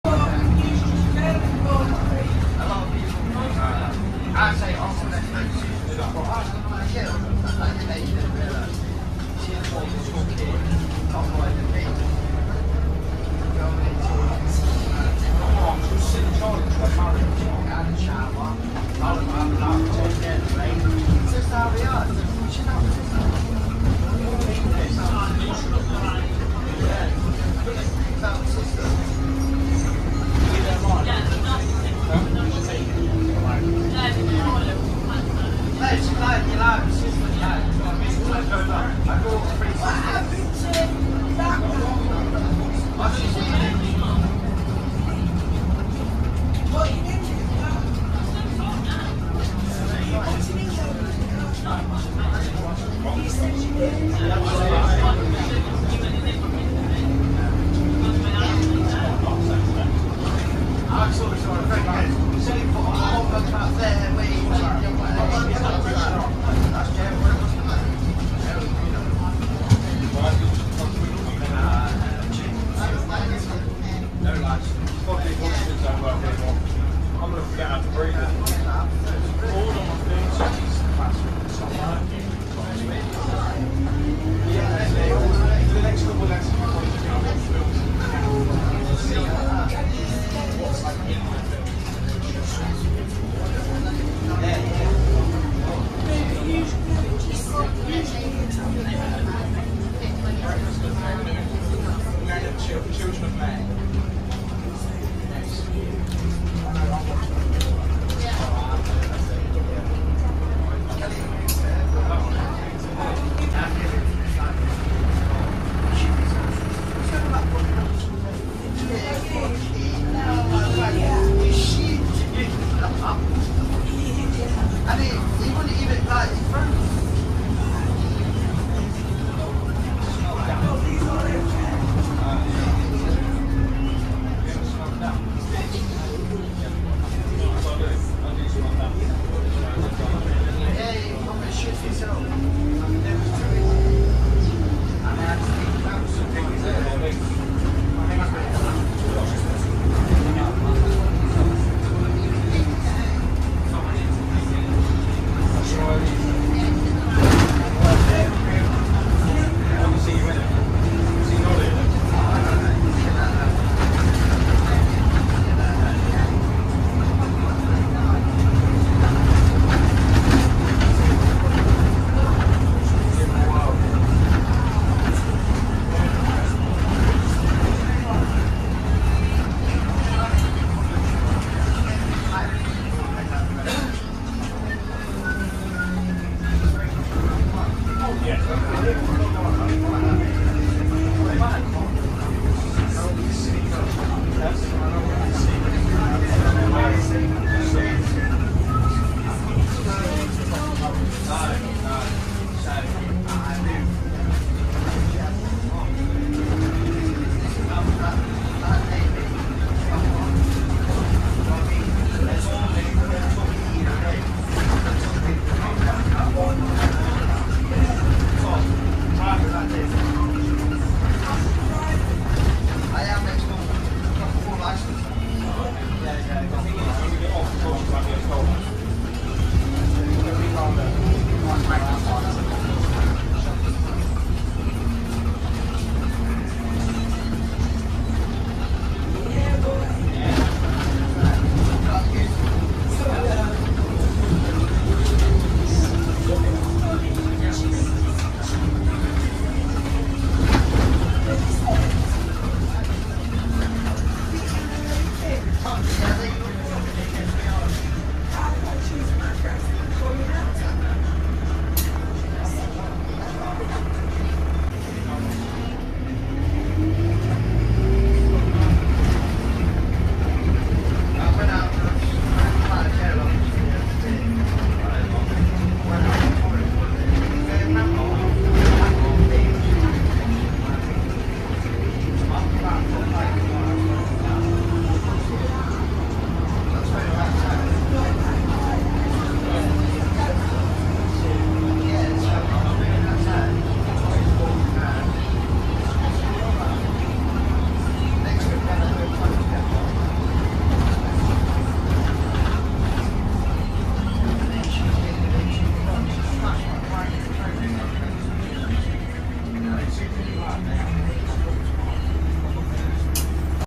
**DIVERSION** Ride on Arriva Midlands ADL Enviro 200MMC 3105 (YX17 NMA) Route 29A
Due to the road closure on Groby Road, routes 27, 28, 29/29A, 125 are diverted via Abbey Gate, Ravensbridge Road, Anstey Lane, Buckminster Road and Brading Road then Groby Road following normal route.

Outbound journeys are not affected. The diversion lasts until November.

Enjoy the diversion :)

**THIS VIDEO IS INTENDED FOR ENTERTAINMENT NOT TRAINING USE**